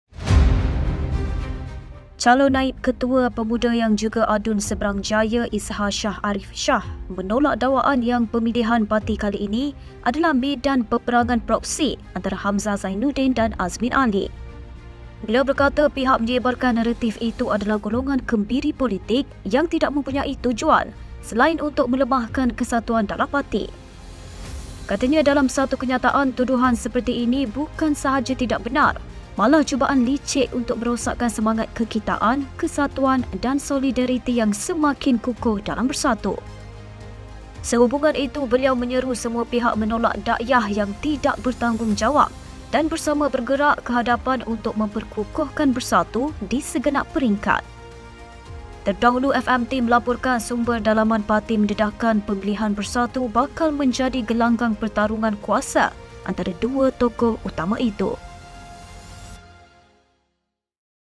‘Kembiri politik sebar dakyah perang proksi’, bidas pemimpin Bersatu
Calon naib ketua Pemuda Bersatu yang juga Adun Seberang Jaya nafi pemilihan parti medan ‘peperangan proksi’.